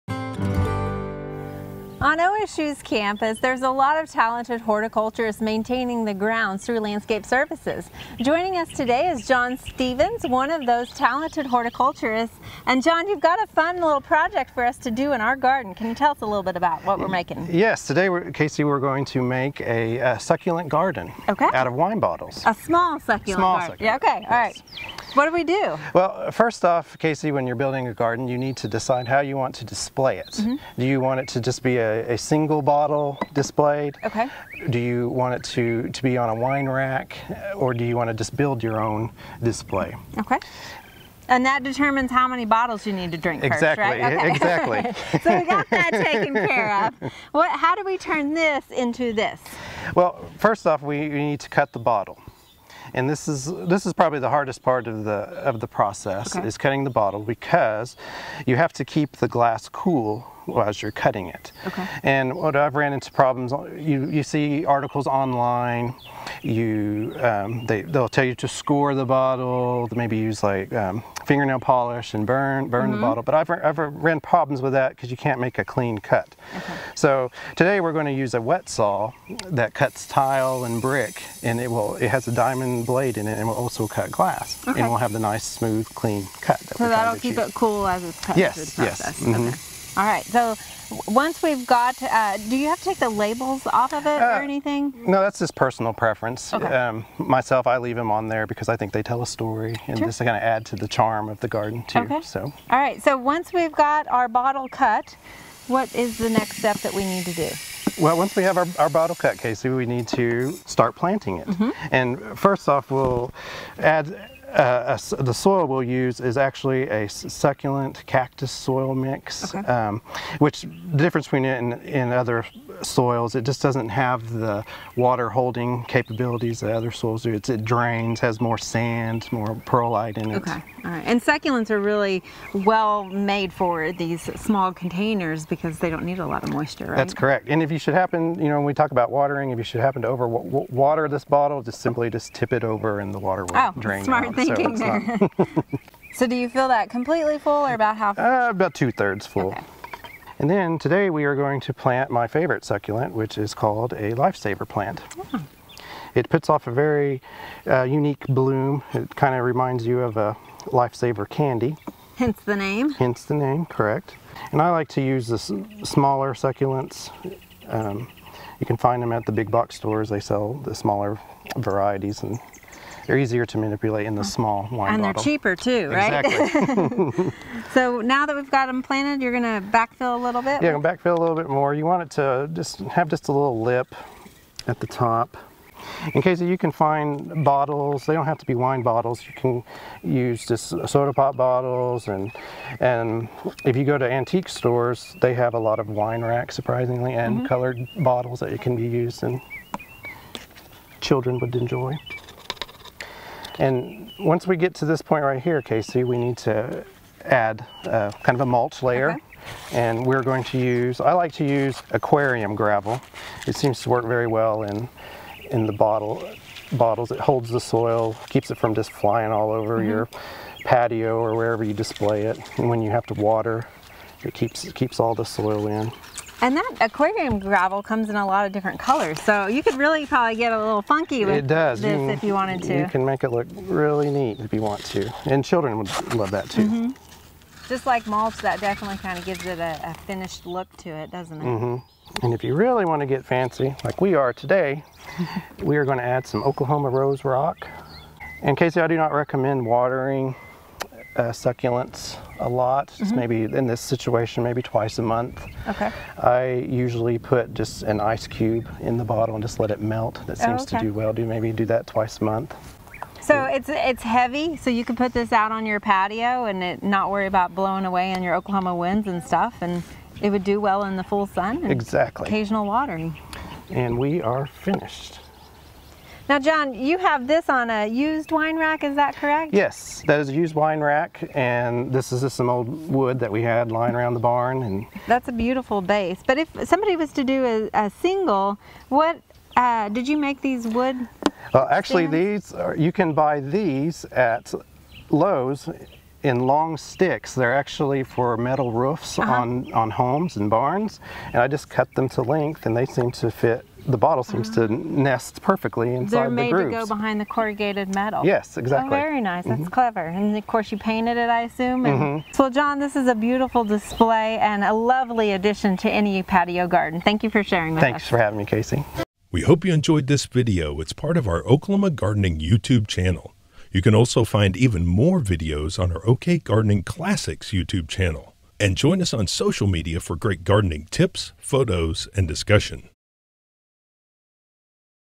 DIY Wine Bottle Succulent Planter
John Stephens, an OSU-Stillwater Horticulturalist, joins Host Casey Hentges to build a succulent planter from a wine bottle.